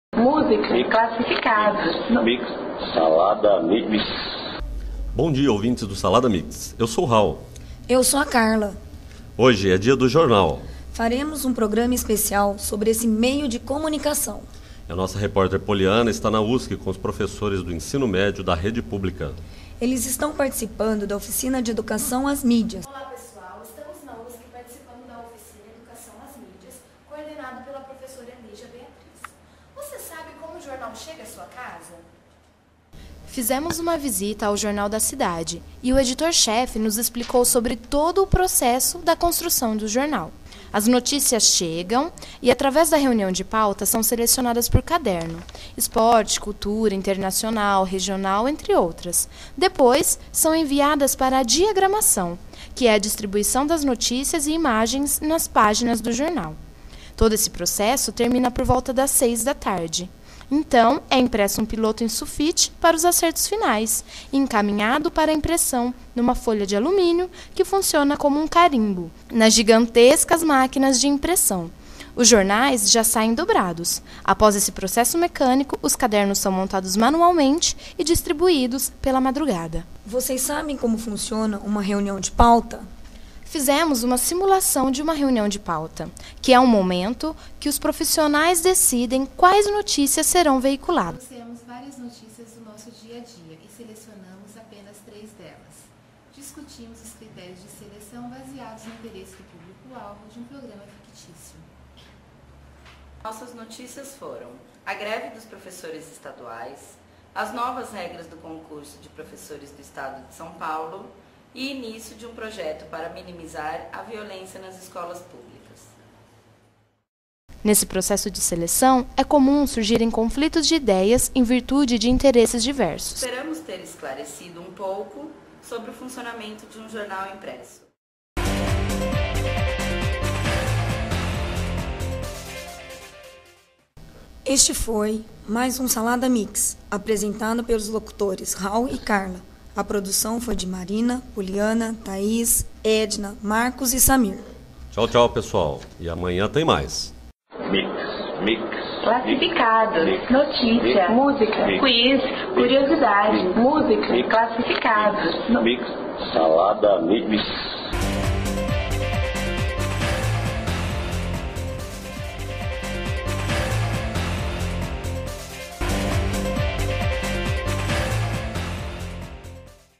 Documentário sobre produção de jornal
Montamos um vídeo a partir de um documentário radiofônico que fizemos falando sobre a forma como é produzido um jornal. Como o material não foi colhido especificamente para produzir um vídeo,  não dava para ficar perfeito.